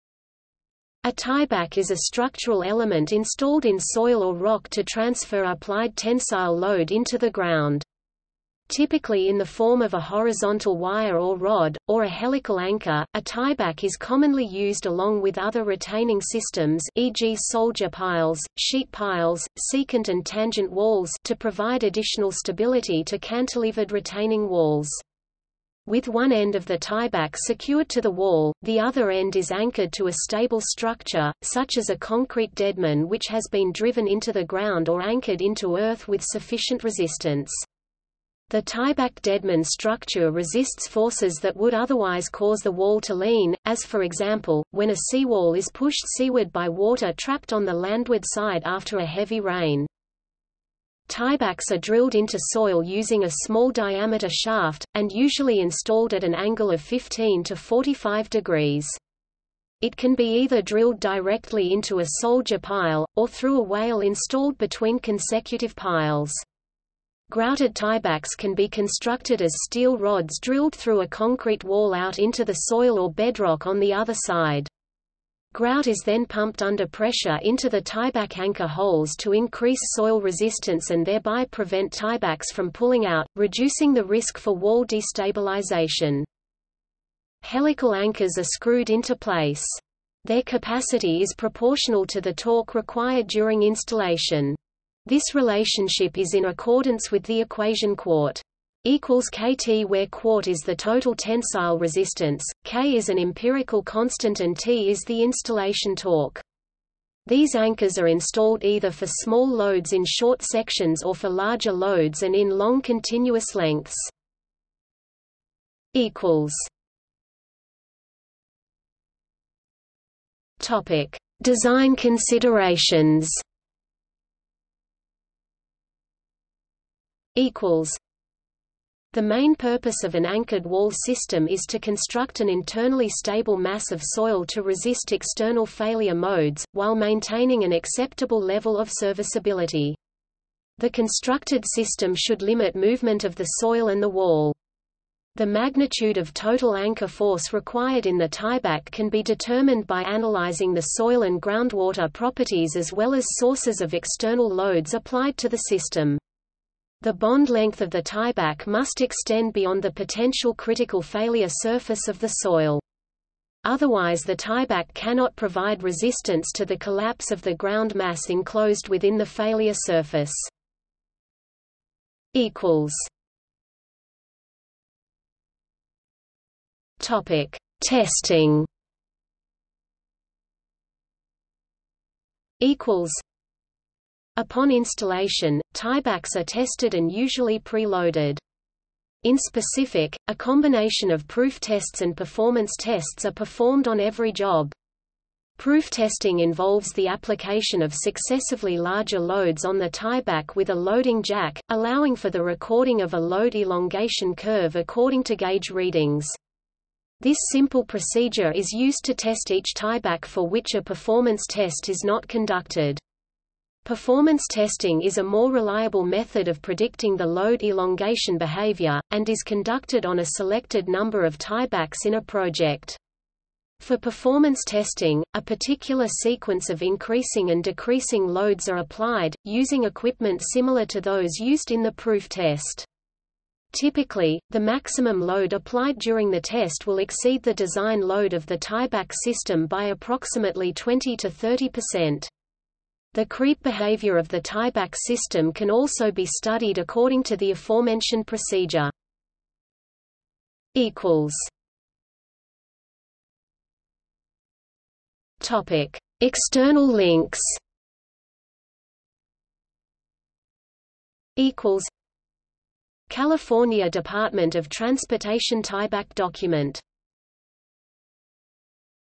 Tieback (geotechnical) | Wikipedia audio article
00:01:38 kT where Qt is the total tensile resistance, k is an empirical constant and T is the installation torque.  These anchors are installed either for small loads in short sections or for larger loads and in long continuous lengths.
00:01:56 Design Considerations
00:02:07 The main purpose of an anchored wall system is to construct an internally stable mass of soil to resist external failure modes, while maintaining an acceptable level of serviceability. The constructed system should limit movement of the soil and the wall. The magnitude of total anchor force required in the tieback can be determined by analyzing the soil and groundwater properties as well as sources of external loads applied to the system. The bond length of the tieback must extend beyond the potential critical failure surface of the soil. Otherwise the tieback cannot provide resistance to the collapse of the ground mass enclosed within the failure surface.
00:02:53 Testing
00:03:02 Upon installation, tiebacks are tested and usually pre-loaded. In specific, a combination of proof tests and performance tests are performed on every job. Proof testing involves the application of successively larger loads on the tieback with a loading jack, allowing for the recording of a load-elongation curve according to gauge readings. This simple procedure is used to test each tieback for which a performance test is not conducted. Performance testing is a more reliable method of predicting the load-elongation behavior, and is conducted on a selected number of tiebacks in a project. For performance testing, a particular sequence of increasing and decreasing loads are applied, using equipment similar to those used in the proof test. Typically, the maximum load applied during the test will exceed the design load of the tieback system by approximately 20 to 30%. The creep behavior of the tieback system can also be studied according to the aforementioned procedure.
00:04:11 External links

- Socrates

SUMMARY
A tieback is a structural element installed in soil or rock to transfer applied tensile load into the ground. Typically in the form of a horizontal wire or rod, or a helical anchor, a tieback is commonly used along with other retaining systems (e.g. soldier piles, sheet piles, secant and tangent walls) to provide additional stability to cantilevered retaining walls . With one end of the tieback secured to the wall, the other end is anchored to a stable structure, such as a concrete deadman which has been driven into the ground or anchored into earth with sufficient resistance. The tieback-deadman structure resists forces that would otherwise cause the wall to lean, as for example, when a seawall is pushed seaward by water trapped on the landward side after a heavy rain.

Tiebacks are drilled into soil using a small diameter shaft, and usually installed at an angle of 15 to 45 degrees . It can be either drilled directly into a soldier pile, or through a wale installed between consecutive piles. Grouted tiebacks can be constructed as steel rods drilled through a concrete wall out into the soil or bedrock on the other side. Grout is then pumped under pressure into the tieback anchor holes to increase soil resistance and thereby prevent tiebacks from pulling out, reducing the risk for wall destabilization.
Helical anchors are screwed into place.  Their capacity is proportional to the torque required during installation.  This relationship is in accordance with the equation Qt = kT where Qt is the total tensile resistance, k is an empirical constant and T is the installation torque.  These anchors are installed either for small loads in short sections or for larger loads and in long continuous lengths.